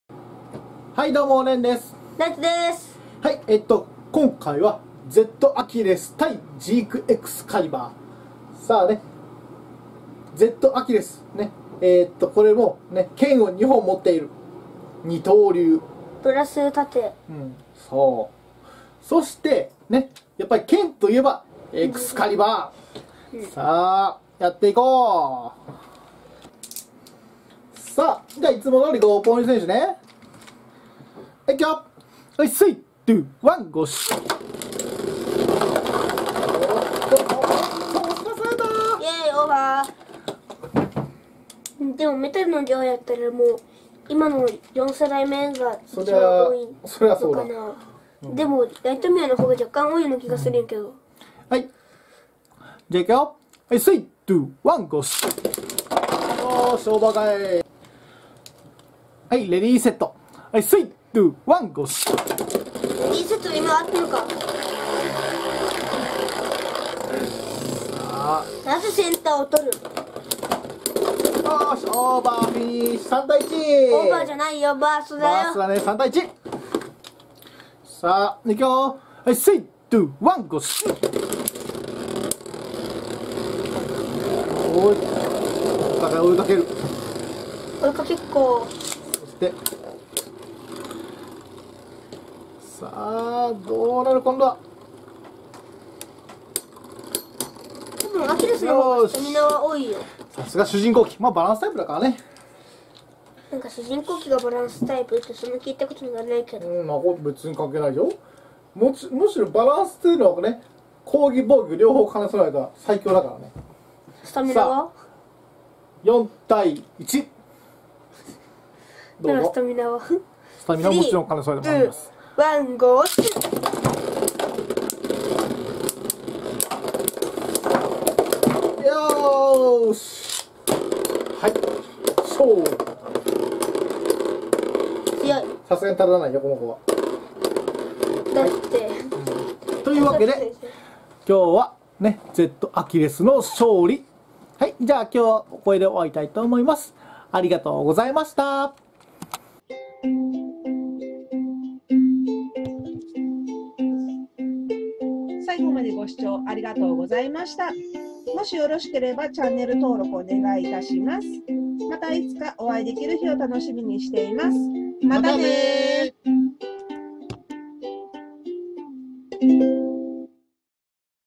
Zアキレスvsジークエクスカリバー 【ベイブレード】
今日のベイ動画は新主人公機
Zアキレスvsジークエクスカリバー
主人公機の力はどれほどのもの？